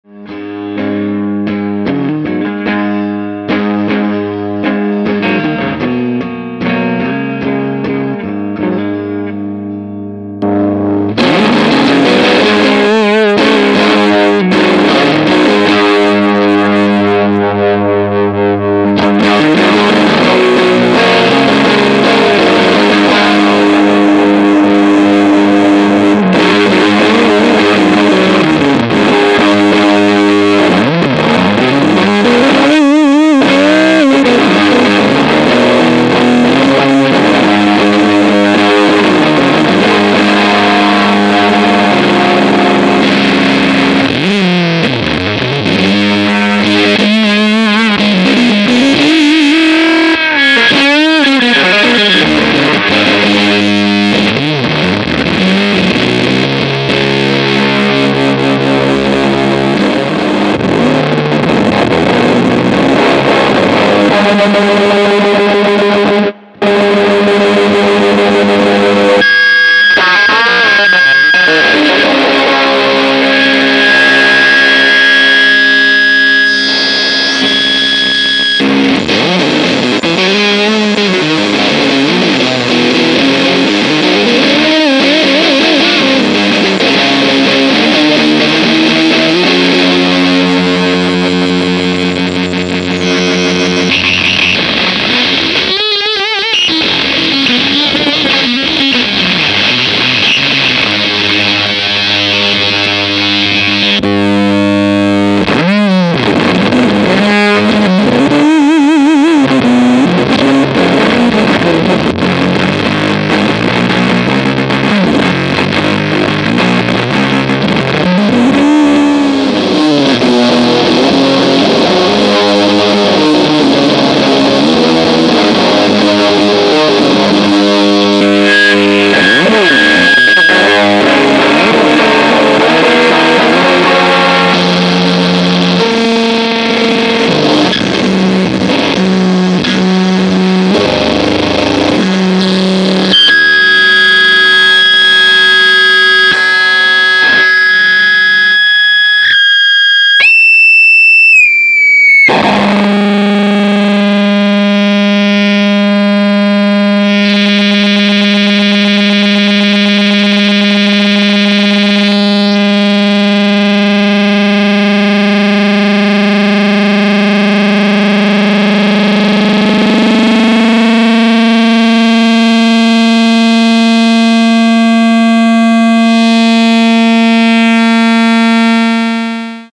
IMPALA DRIVES (Kevin Parker, Tame Impala inspired pedal)
After an incredible night at a TAME IMPALA concert I decided to make a pedal based on the most characteristic sounds of Kevin Parker's pedalboard.
From this idea comes IMPALA DRIVES, a limited series of only 3 pedals.
On the one hand a highly modified Blues Driver, more gain, more tone and a saturation from soft and mellow to much more aggressive.
On the other hand, the perfect combination between the wonderful Companion Fuzz and the famous Fuzz Face. A powerful and ripping sound, with pitch control straight out of favorite BMP.
Switch with a deeper octave option and drone synth in the fuzz and another switch where you can rearrange the order of the effects to achieve an infinite sonic range.
A unique and majestic pedal like the music of Tame Impala.